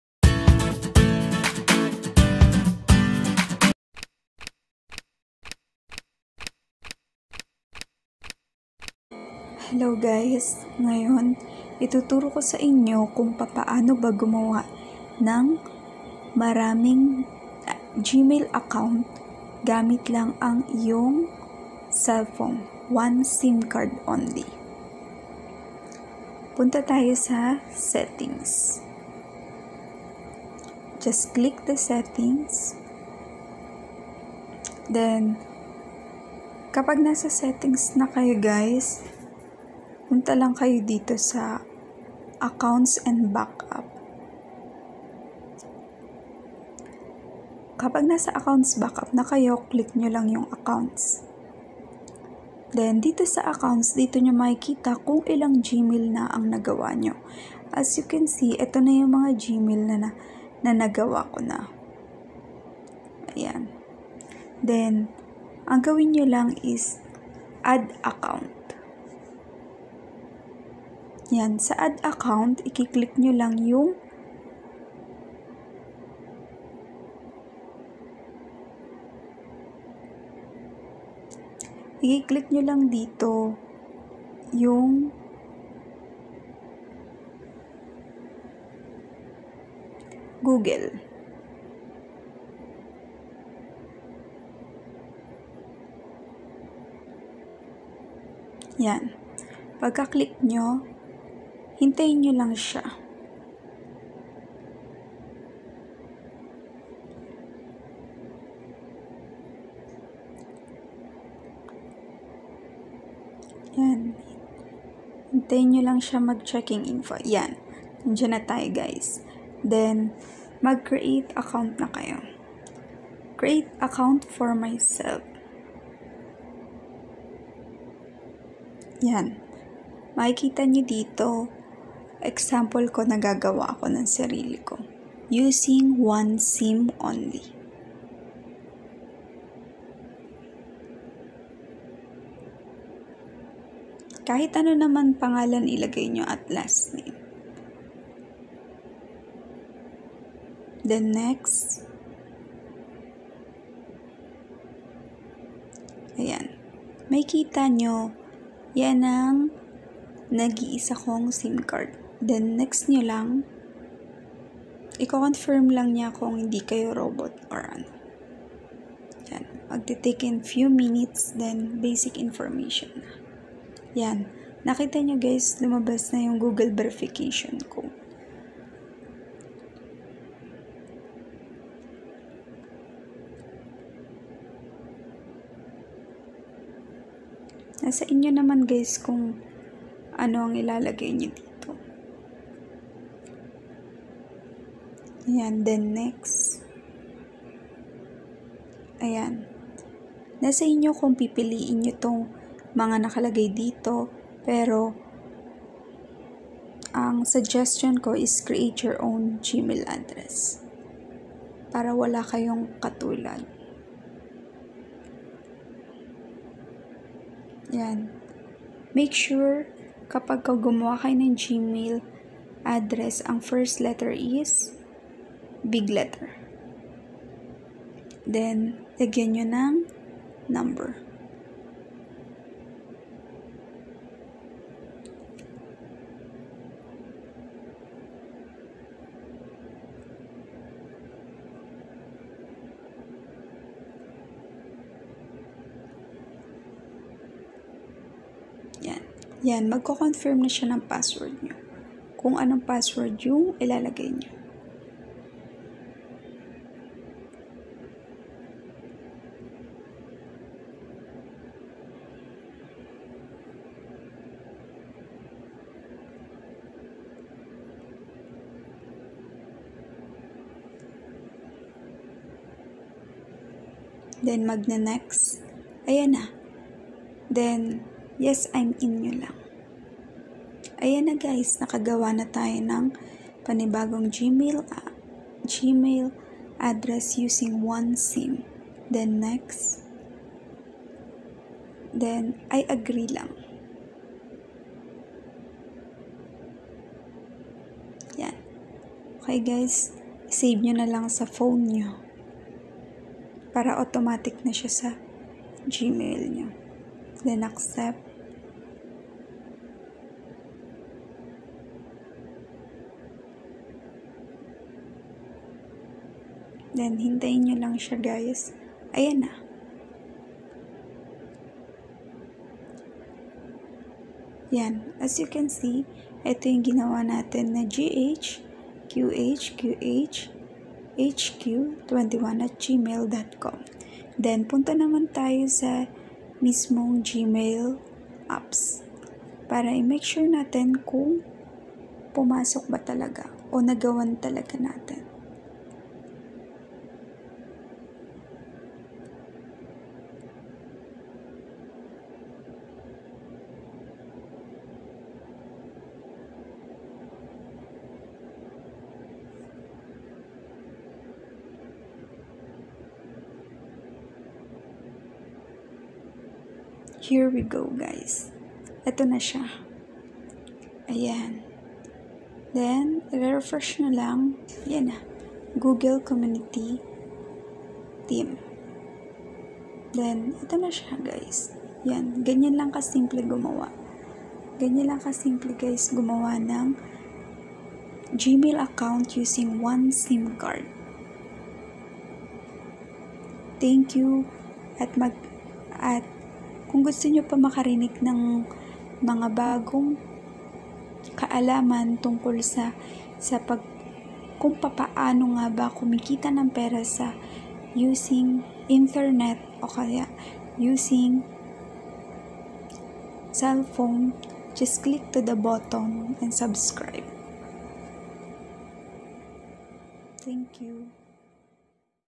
Create gmail account
Creat Gmail Account

•Para sa mga di nakakaalam kung papaanu gumawa ng gmail account using one sim card